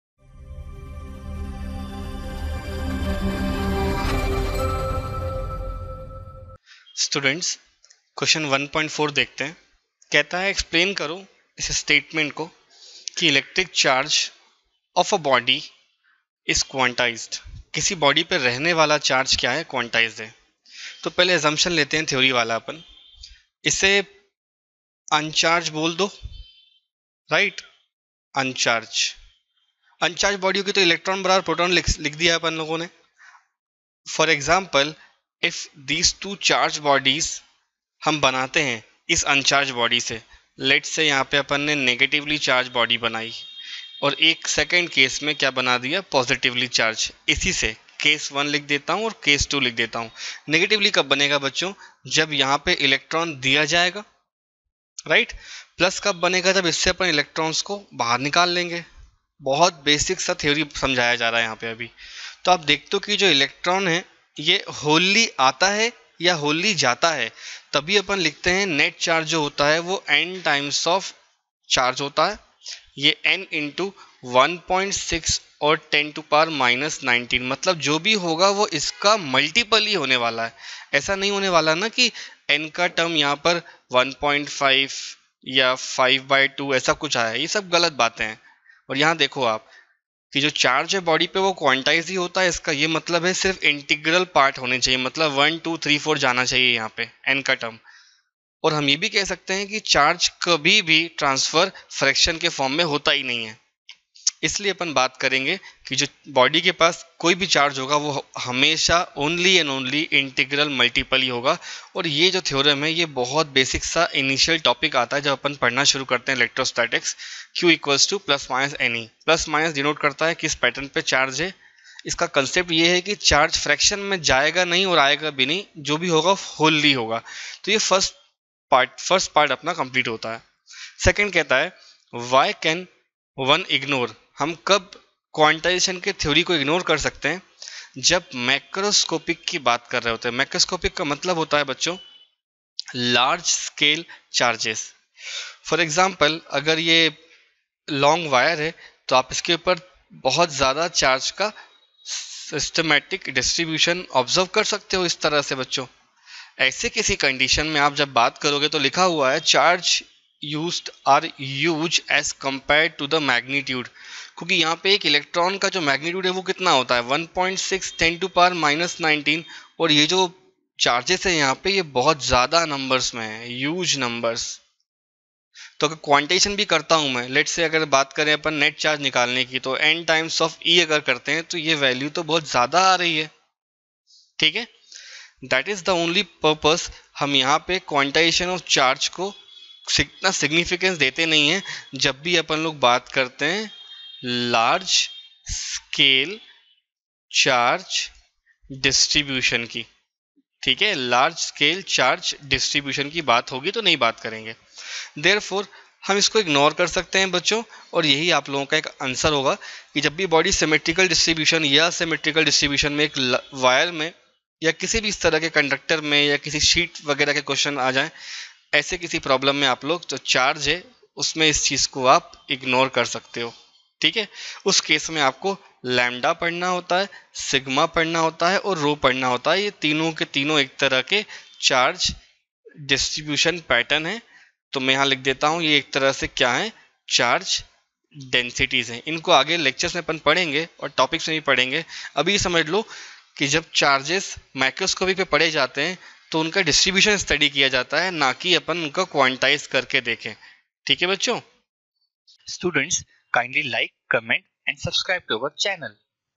NCERT Solutions Class 12 Physics | Chapter - 1 Electric Charges and Fields Ex 1.4
Content of this video:
4. (a) Explain the meaning of the statement ‘electric charge of a body is quantised’.
(b) Why can one ignore quantisation of electric charge when dealing with macroscopic i.e., large scale charges?

Video Lectures for class 9 to 12, Dropper for preparing JEE/NEET/AIIMS/KVPY.